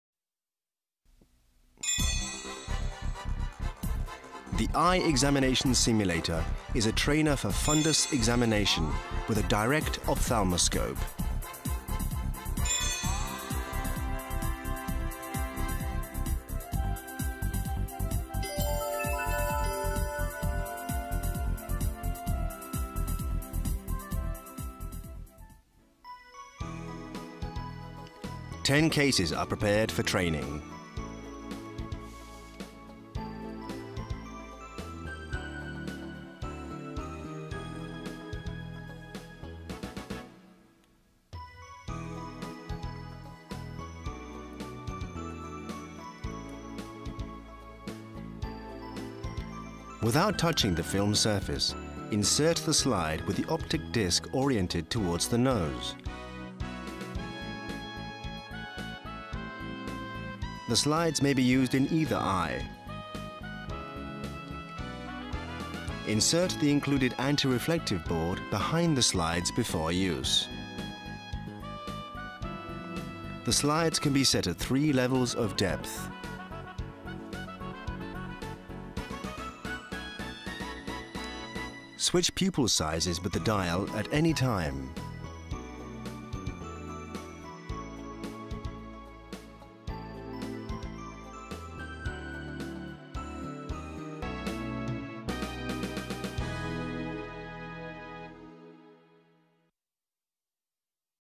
Exame de olho avançado - M82
este simulador avançado para exame de olho inovador é destinado ao treinamento de exame de fundo de olho com oftalmoscópio